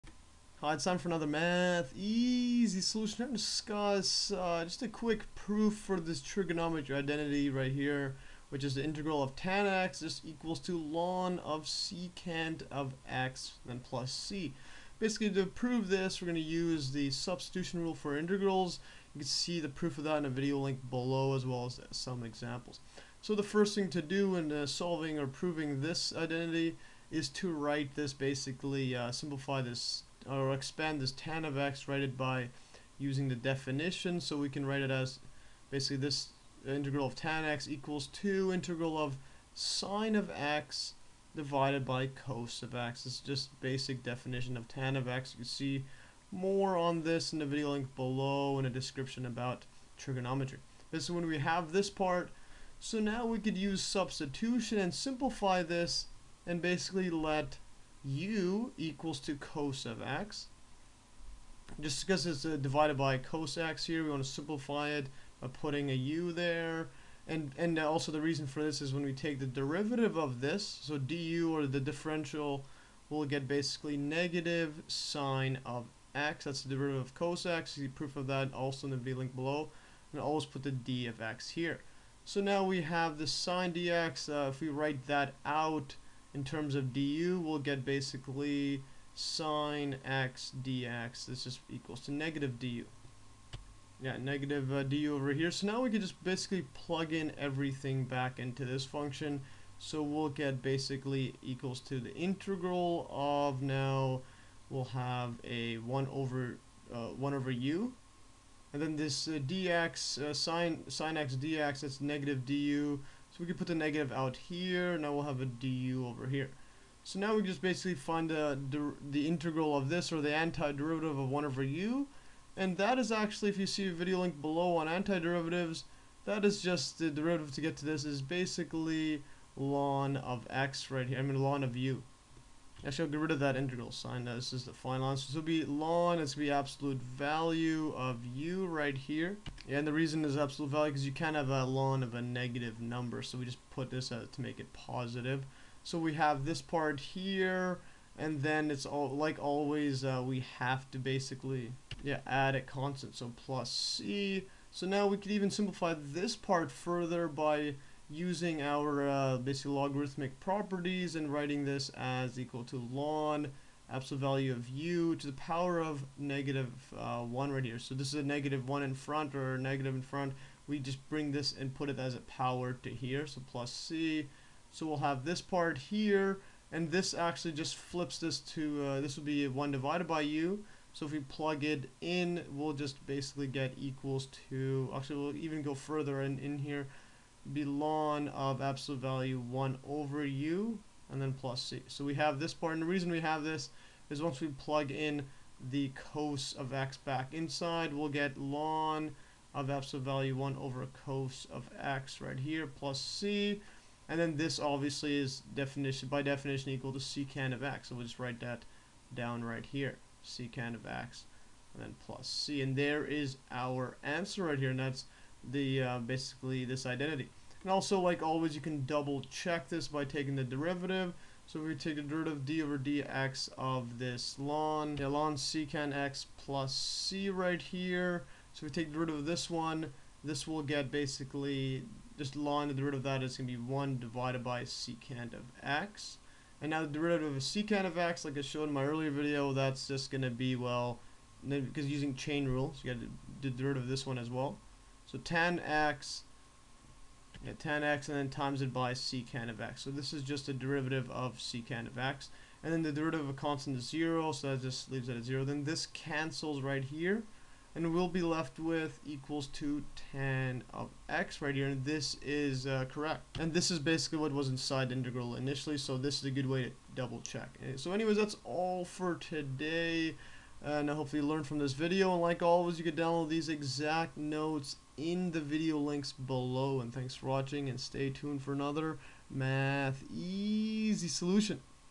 Trigonometry Identity: Integral of tan(x)
In this video I go over proving the trigonometry Identity of the integral of tan(x) = ln|sec(x)| + C. In the proof I use the Substitution Rule for integrals to change the variable from x to u.